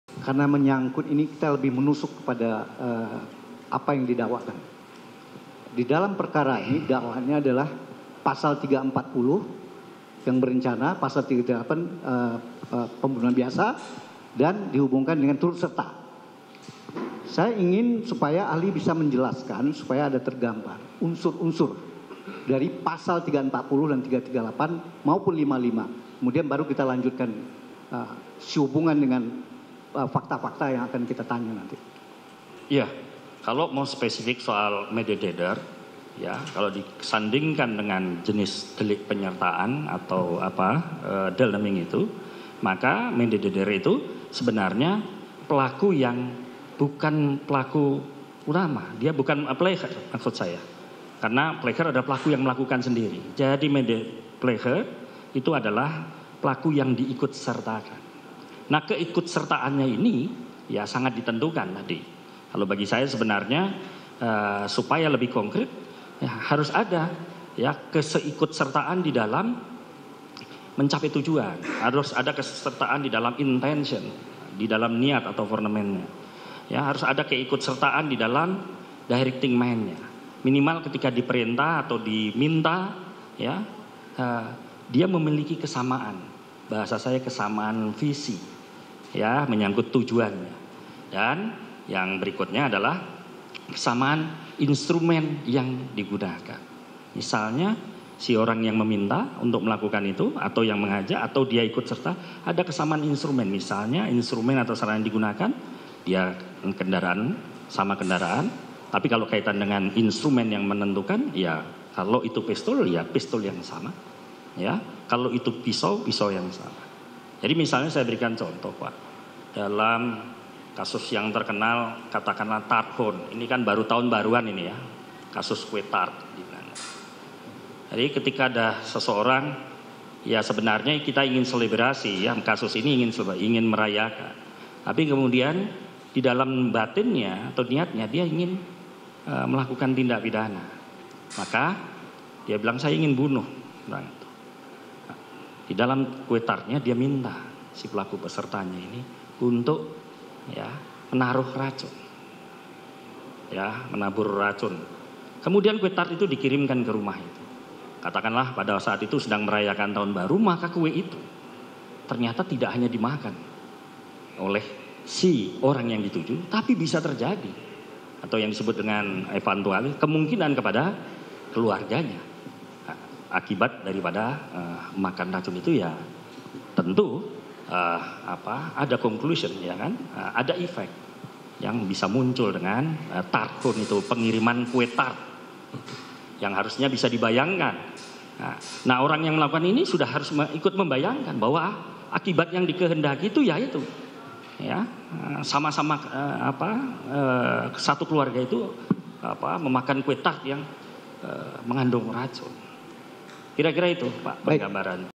Jelaskan Instrumen Pembunuhan Berencana, Ahli Meringankan Ricky: Harus Ada Kesamaan Niat dan Visi
JAKARTA, KOMPAS.TV - Terdakwa pembunuhan Brigadir Yosua, Ricky Rizal menjalani sidang lanjutan hari ini (04/01).

Sidang hari ini diagendakan untuk mendengarkan keterangan dari ahli untuk meringankan tuntutan jaksa.

Dua ahli yang dihadirkan tim Kuasa Hukum Ricky Rizal hari ini adalah Firman Wijaya, Ahli Hukum Pidana Dari Universitas Krisnadwipayana dan Solahudin, Ahli Hukum Pidana dari Universitas Bhayangkara (Ubhara) Surabaya.

Ahli Hukum Pidana Universitas Tarumanegara, Firman Wijaya menyatakan instrumen utama dalam kasus pembunuhan berencana.

Yaitu harus ada kesamaan niat dan adanya keikutsertaan atau ikut berpartisipasi dalam kejahatan itu.

Selain itu, harus ada juga kesamaan instrument atau alat yang digunakan.